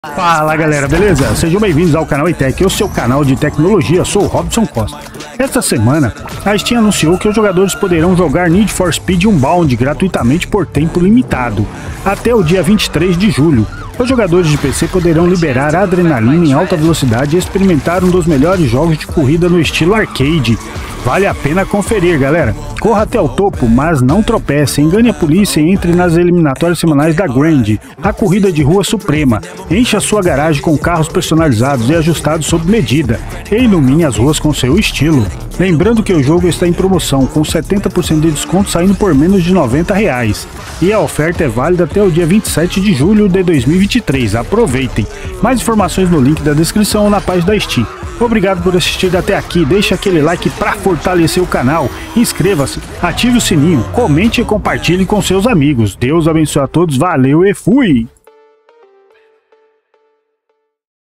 Need For Speed Unbound na Steam Menos 48H Liberado pra Jogar!
Fala galera beleza! Esta semana, a Steam anunciou que os jogadores poderão jogar Need for Speed Unbound gratuitamente por tempo limitado, até o dia 23 de Julho de 2023, os jogadores de PC poderão jogar gratuitamente na Steam. Confiram!
Corra até o topo, mas não tropece. Engane a polícia e entre nas eliminatórias semanais do Grand, a corrida de rua suprema. Encha sua garagem com carros personalizados e ajustados sob medida, e ilumine as ruas com seu estilo.
Mais informações no link da descrição ou na página da Steam.

Créditos/Notícias/Fonte:

Tweet: Need for Speed - @NeedforSpeed

Confira abaixo os requisitos técnicos para rodar o jogo no PC:

Mínimos

SO: Windows 10 64-bit
Processador: Ryzen 5 2600, Core i5-8600
Memória: 8 GB de RAM
Placa de vídeo: RX 570, GTX 1050 Ti
DirectX: Versão 12
Rede: Conexão de internet banda larga
Armazenamento: 50 GB de espaço disponível
Recomendados

Processador: Ryzen 5 3600, Core i7-8700
Memória: 16 GB de RAM
Placa de vídeo: Radeon RX5700 (8GB), GeForce RTX 2070 (8GB)
DirectX: Versão 12

PROMOÇÃO ESPECIAL! Oferta válida até 27 de Julho de 2023.